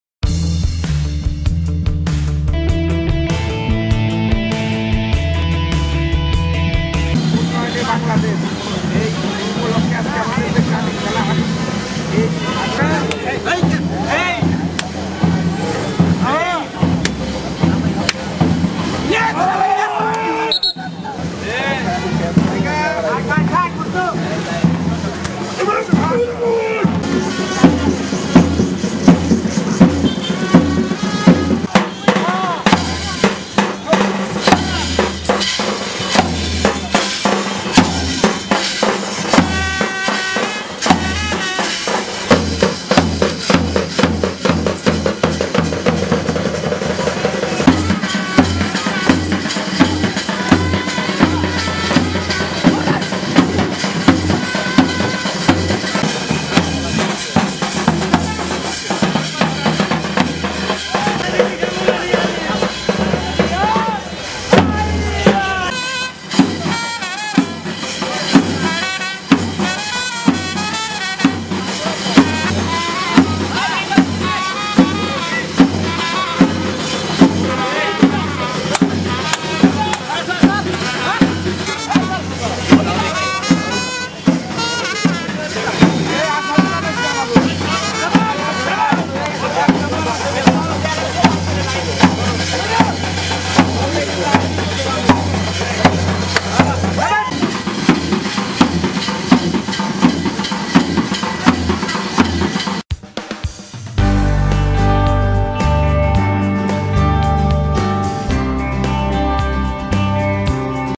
টাঙ্গাইলে ঐতিহ্যবাহী হা-ডু-ডু এবং লাঠি খেলা
টাঙ্গাইলে  শনিবার বিকেলে গ্রাম-বাংলার ঐতিহ্যবাহী হা-ডু-ডু এবং লাঠি খেলা অনুষ্ঠিত হয়েছ্ ে টাঙ্গাইল জেলা ক্রীড়া সংস্থা স্থানীয় শহীদ স্মৃতি পৌর উদ্যানে এ খেলাধুলার আয়োজন করে।